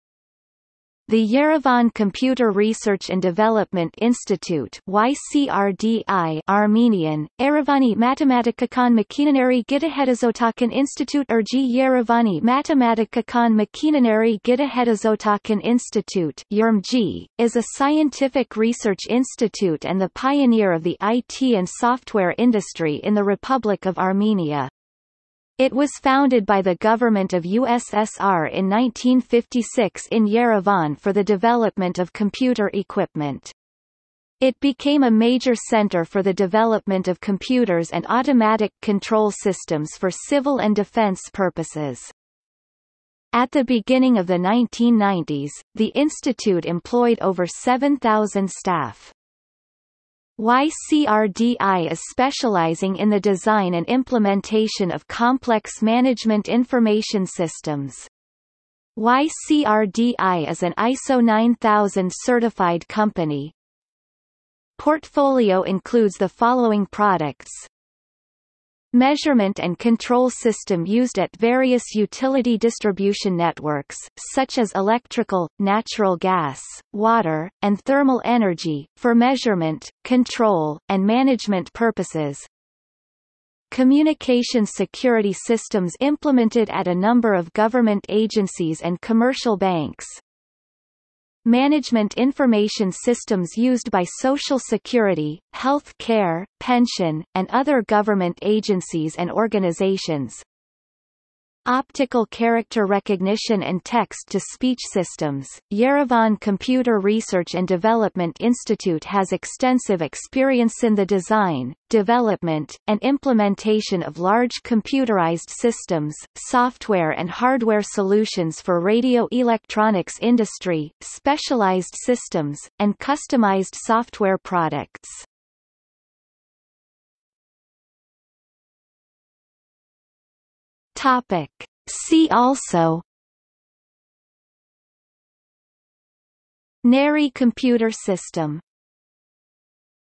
Yerevan Computer Research and Development Institute | Wikipedia audio article
00:02:07 See also

- Socrates

SUMMARY
The Yerevan Computer Research and Development Institute (YCRDI) (Armenian: Երևանի մաթեմատիկական մեքենաների գիտահետազոտական ինստիտուտ (ԵրՄՄԳՀԻ) (Yerevani mat'ematikakan mekenaneri gitahetazotakan institut (YerMMGHI))), is a scientific research institute and the pioneer of the IT and software industry in the Republic of Armenia. It was founded by the government of USSR in 1956 in Yerevan for the development of computer equipment. It became a major centre for the development of computers and automatic control systems for civil and defense purposes.
At the beginning of the 1990s, the institute employed over 7,000 staff.
YCRDI  is specializing in the design and implementation of complex management information systems. YCRDI is an ISO 9000 certified company.
Portfolio includes the following products:

Measurement and control system used at various utility distribution networks, such as electrical, natural gas, water, and thermal energy, for measurement, control, and management purposes;
Communication security systems implemented at a number of government agencies and commercial banks;
Management information systems used by social security, health care, pension, and other government agencies and organizations;
Optical character recognition and text-to-speech systems.Yerevan Computer Research and Development Institute has extensive experiencein the design, development, and implementation of large computerized systems, software and hardware solutions for radio electronics industry, specialized systems, and customized software products.